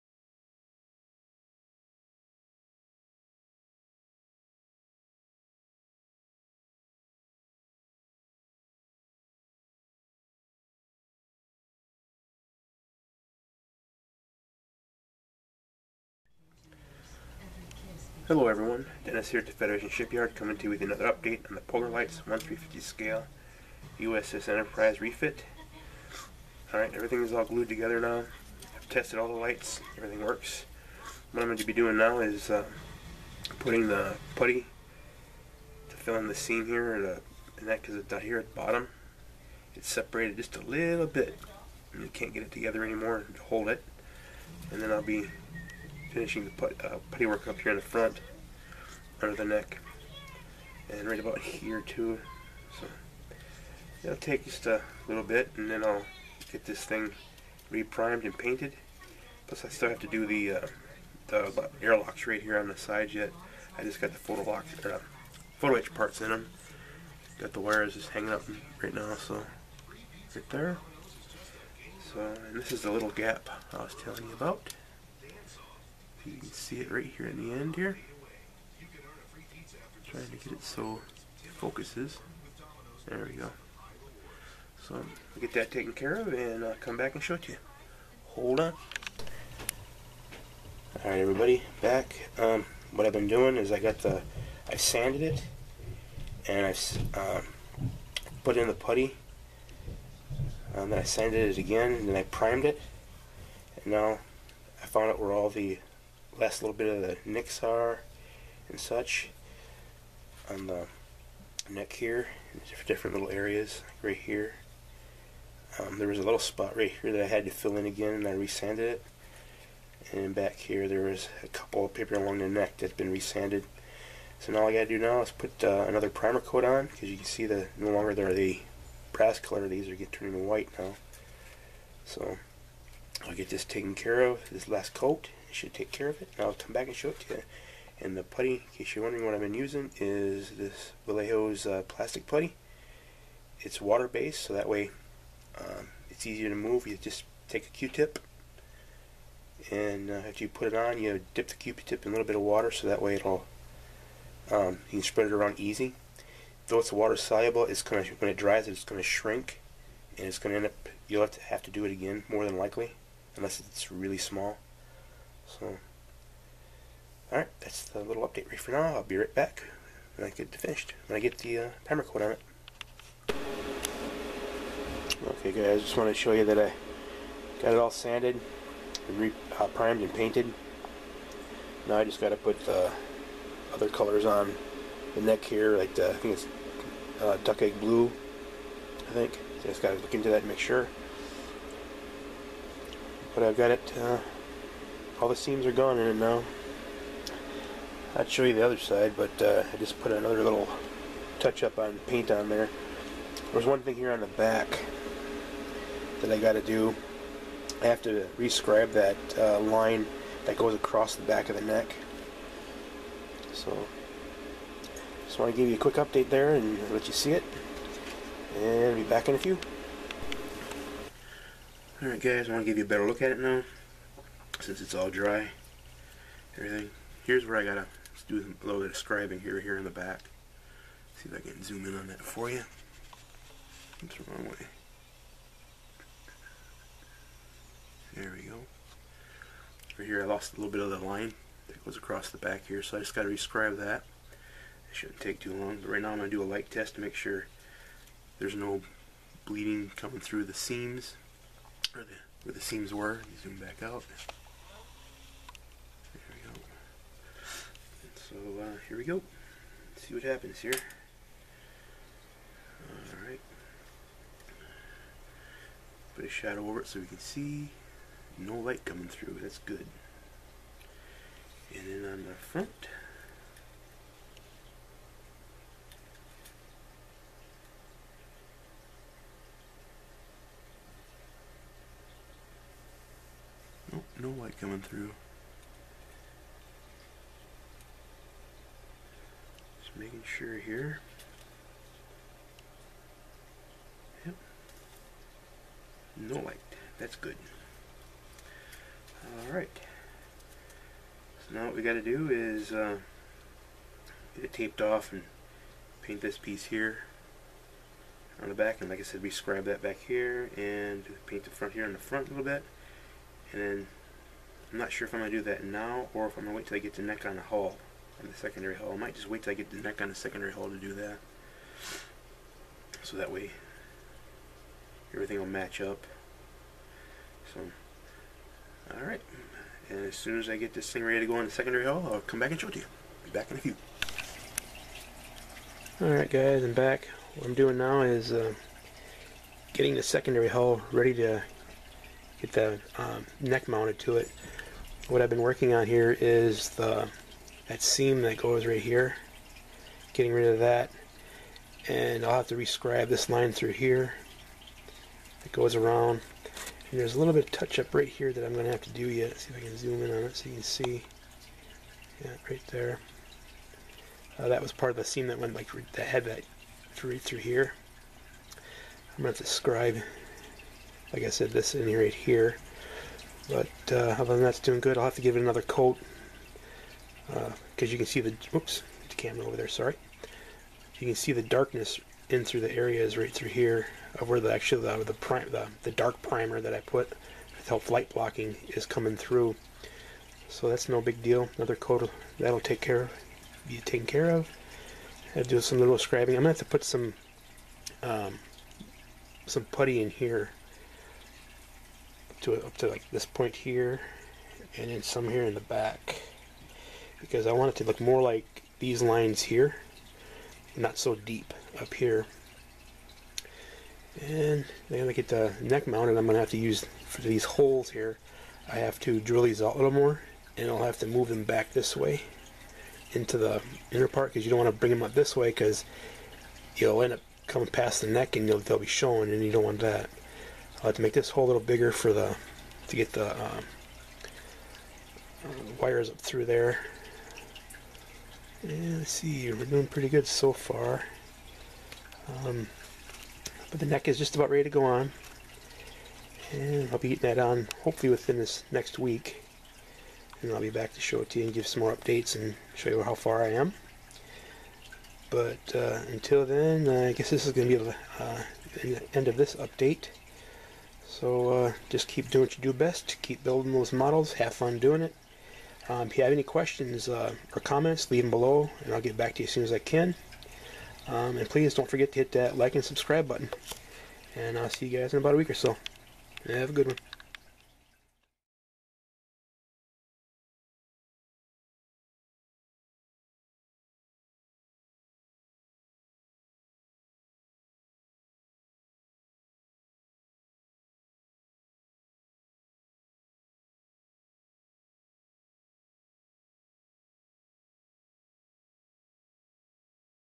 POLAR LIGHTS 1/350 SCALE ENTERPRISE REFIT BUILD PT. 24
Doing the seem work on the neck of the Enterprise and some painting and then getting the secondary hull ready to have the neck mounted on.